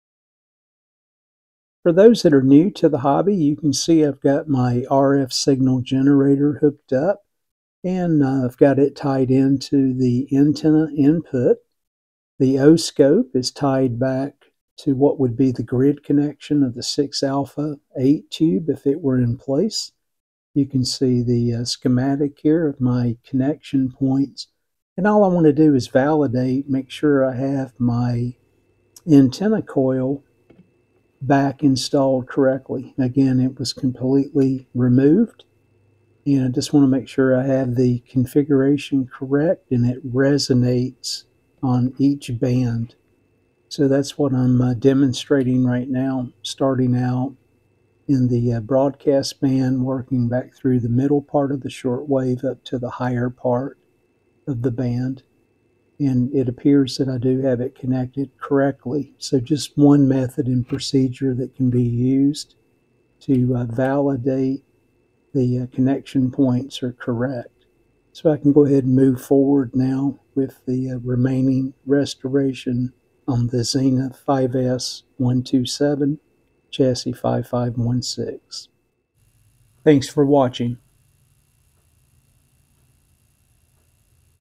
Antenna Coil Connectivity Validation Tip
For folks that are new to the hobby, a simple but effective way to validate the antenna coil is connected properly and working as it should. In my case, I had removed the antenna coil from the circuit and cleaned up the wire dress issues, etc., for the Zenith 5-S-127, chassis 5516 from 1937.